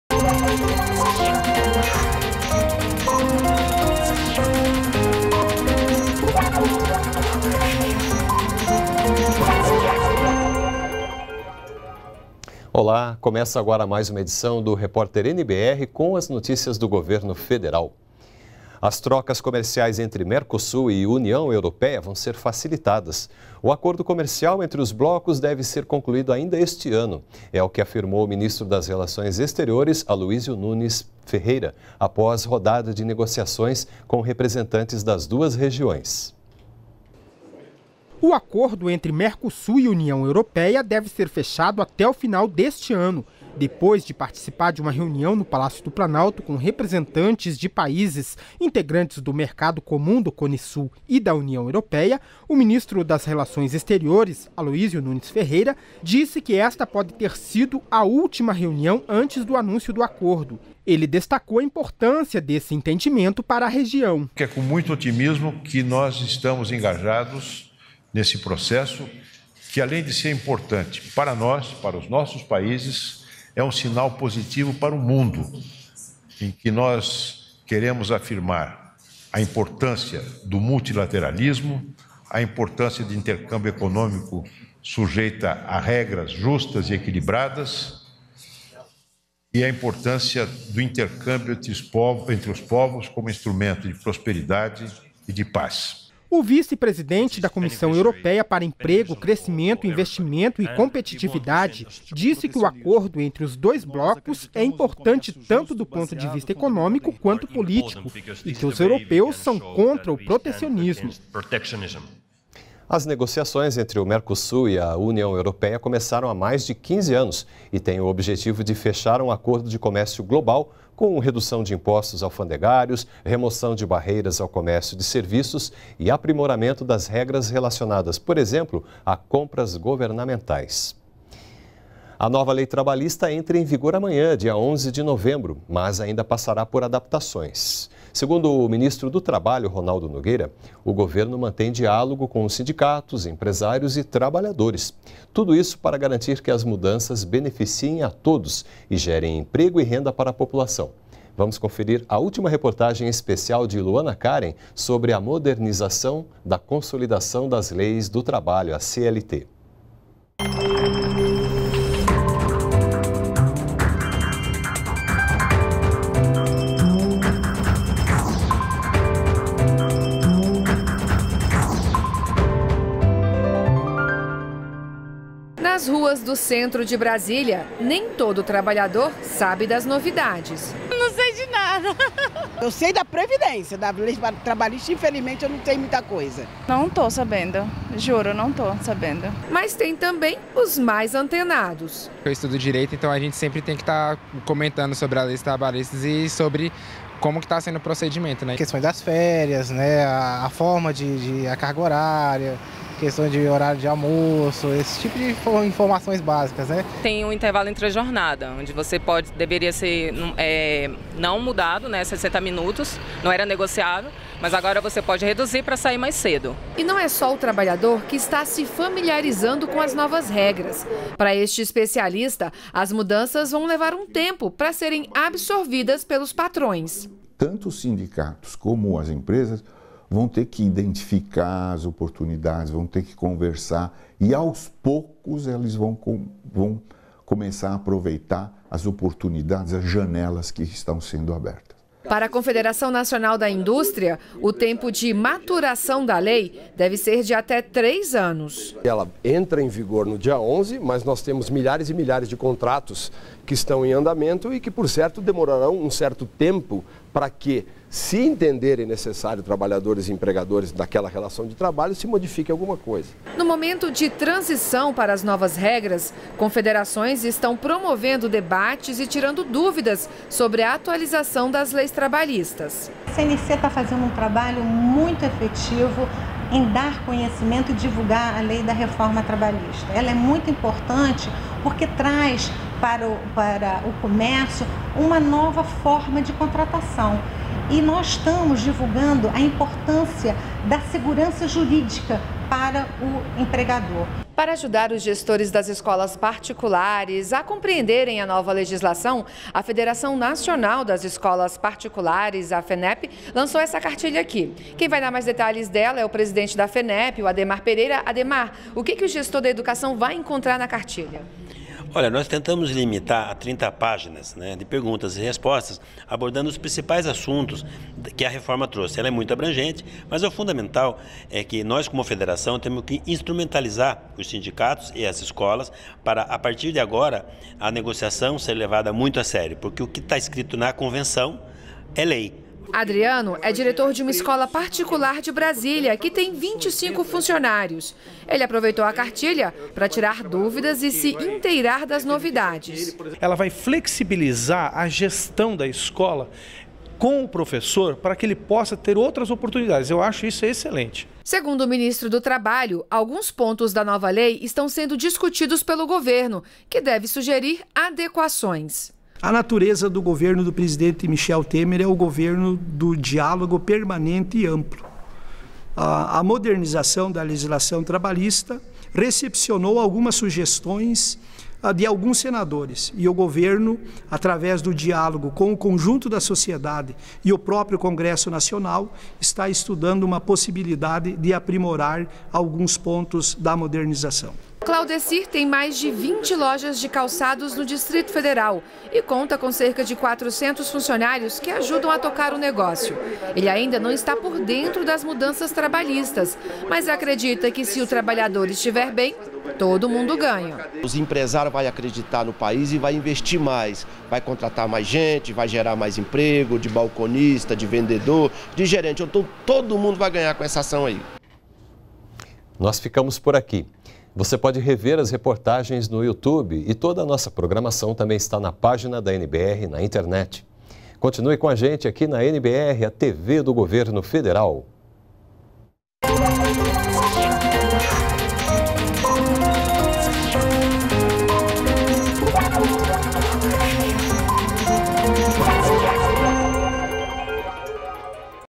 BOLETIM REPÓRTER NBR 17h - 10 de novembro de 2017
REPÓRTER NBR 17h - 10.11.17:  As trocas comerciais entre Mercosul e União Europeia vão ser facilitadas. O acordo comercial entre os blocos deve ser concluído ainda este ano. E mais: A nova Lei Trabalhista entra em vigor em 11 de novembro, mas ainda passará por adaptações.  Segundo o Ministro do Trabalho, Ronaldo Nogueira, o governo mantém diálogo com os sindicatos, empresários e trabalhadores para garantir que as mudanças beneficiem a todos e gerem emprego e renda para a população.